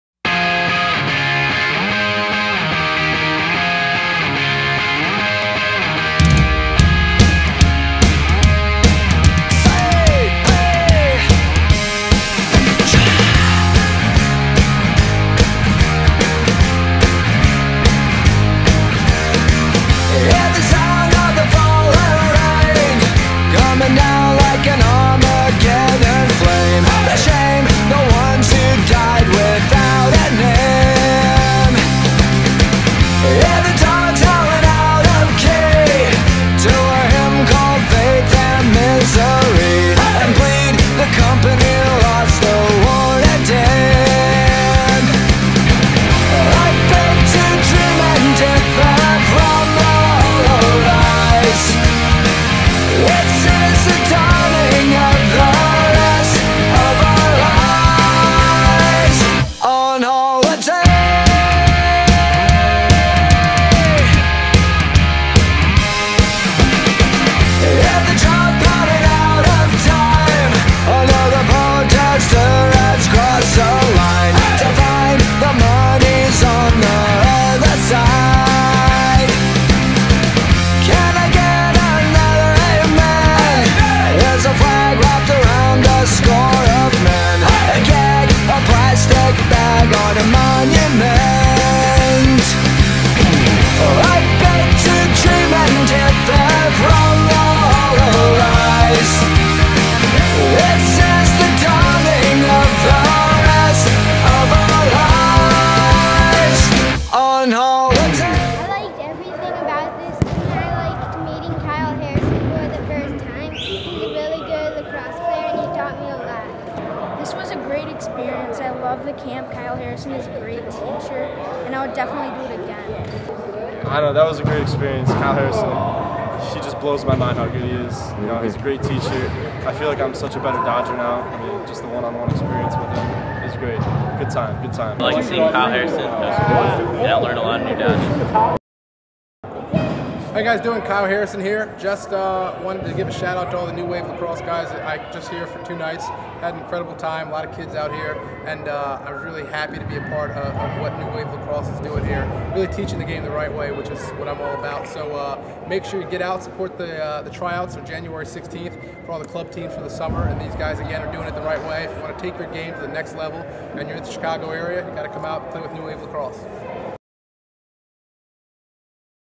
LXM Pro Kyle Harrison with New Wave Lacrosse
One of lacrosse's biggest stars teaches young lacrosse players some new skills. He also gives a huge shout out to the New Wave Tryouts on January 16, 2012